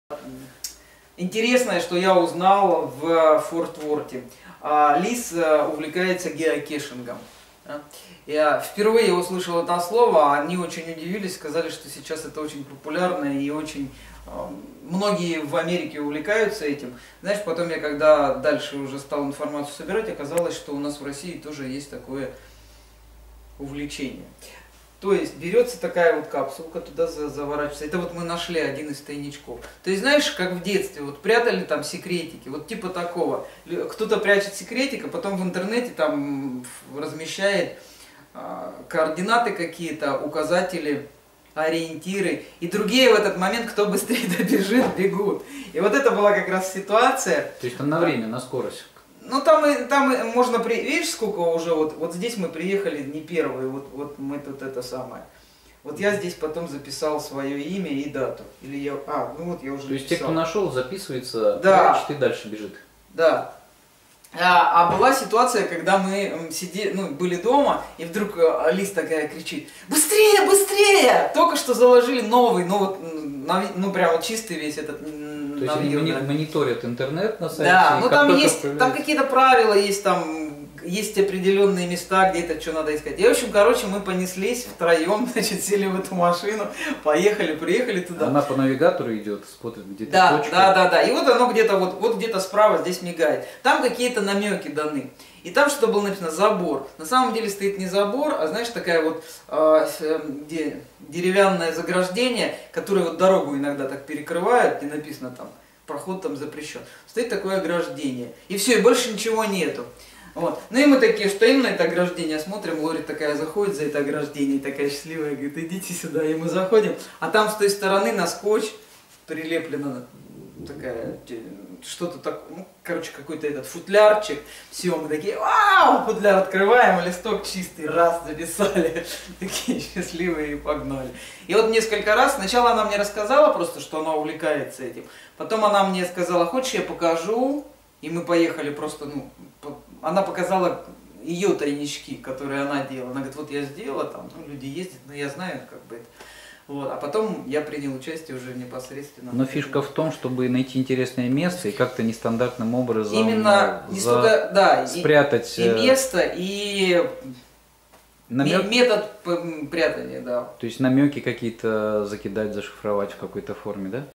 Александр Удалов, 52. Геокешинг в Америке, Форт Уорт, Кругосветка 2013-14
АЛЕКСАНДР УДАЛОВ, АВТОСТОПОМ ВОКРУГ ПЛАНЕТЫ, ЖИЗНЬ БЕЗ ГАРАНТИЙ, ЭКСПЕРИМЕНТ-ИССЛЕДОВАНИЕ, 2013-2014.
ALEKSANDR UDALOV, HITCHHIKER AROUND THE PLANET, LIFE WITHOUT GUARRANTEES. THE EXPERIMENT - EXPLORING, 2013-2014.